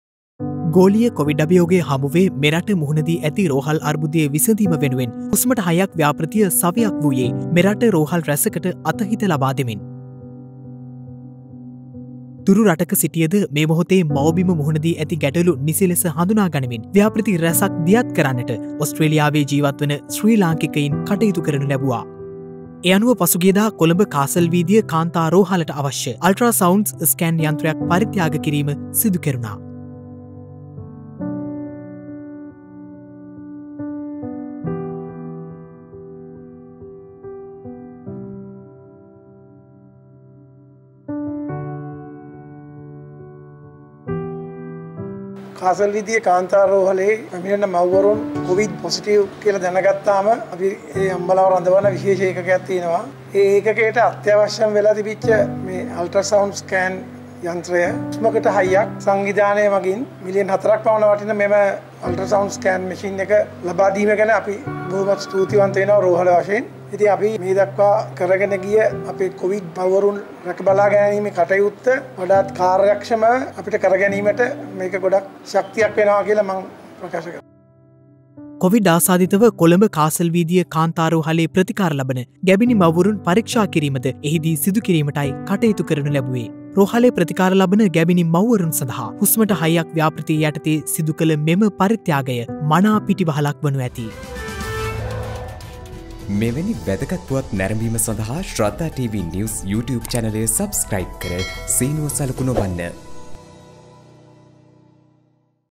හුස්ම නැති වන්නට නොදී රැකගනු පිණිස හුස්මට හයියක්...(2021-07-30)
මෙරට පැතිර යන කොවිඞ් වසංගතය හේතුවෙන් රෝහල් පද්ධතිය මුහුණ දී ඇති ගැටලූ ස`දහා සරණක් වෙමින්  ඕස්ටේ‍්‍රලියාවේ වෙසෙන ශ‍්‍රී ලාංකිකයින් පිරිසක් එක්ව පරිත්‍යාග රැසක් සිදුකර තිබෙනවා.

ඒ හුස්මට හයියක් ව්‍යාපෘතිය යටතේයි.

එහි තවත් අදියරක් පසුගිය දා පැවැත්වුනේ කොළඹ කාසල් රෝහල කේන්ද්‍ර කර ගනිමින්.